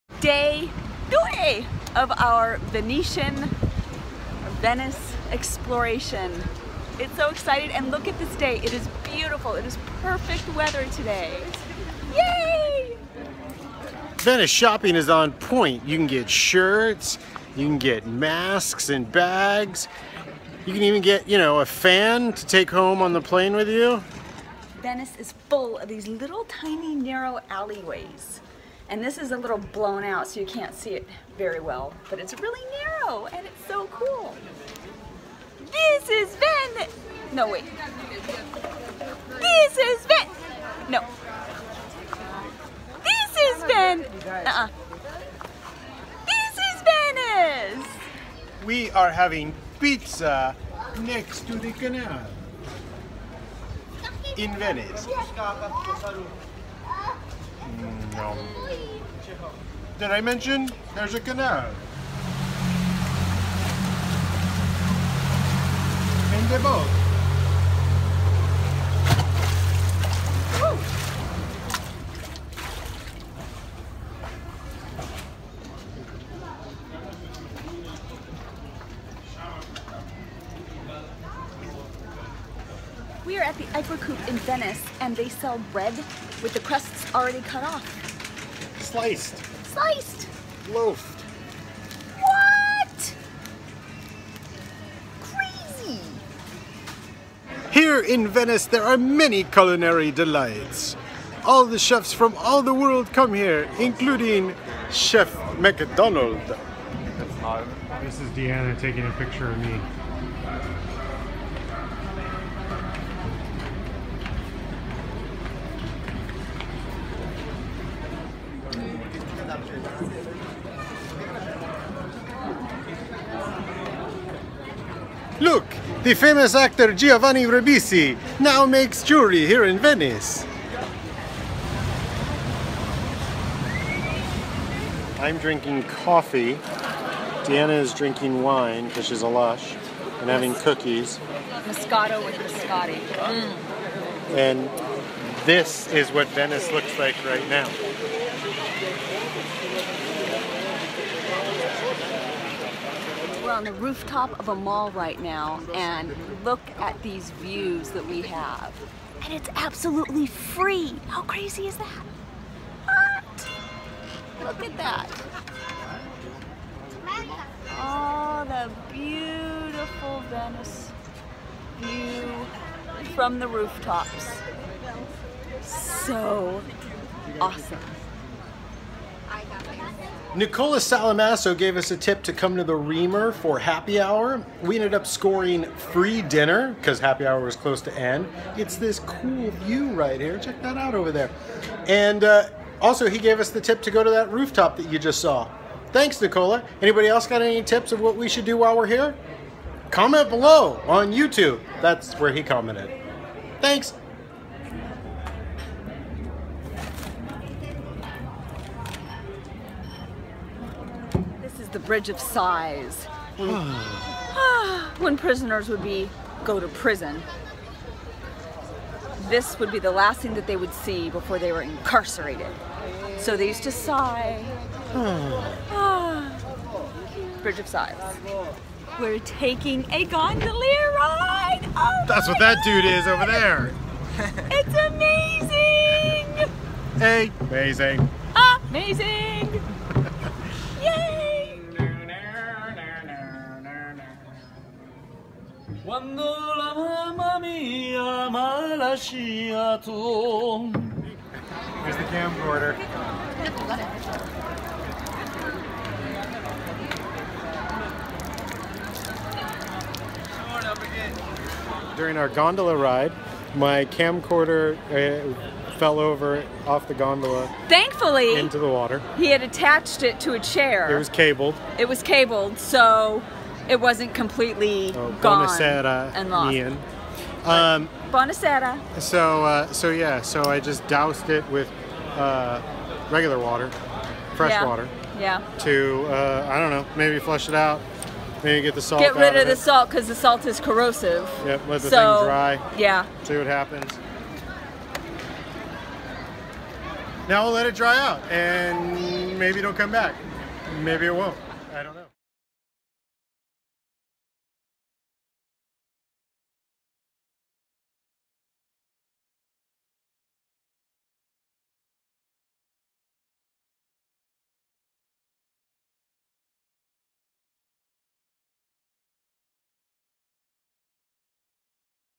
20 Exploring Venice Italy VLOG
Adventure through Venice aka Venezia Italy with us as we walk the streets and take a gondola ride to discover a singing gondolier.